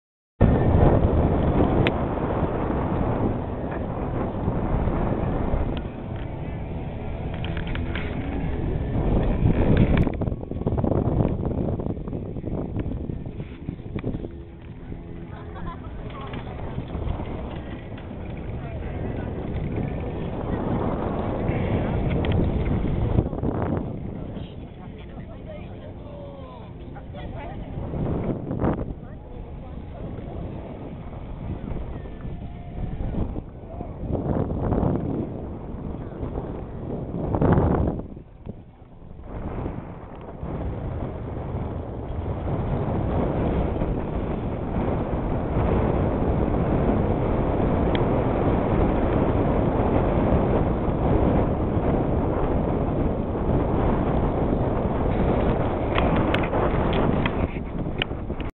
Burning Man 2004 - Dust Devils on the Playa
The day was bright and clear but not too windy. This was the first break after about 2 days of steady 50+ mph winds (which took down our dome).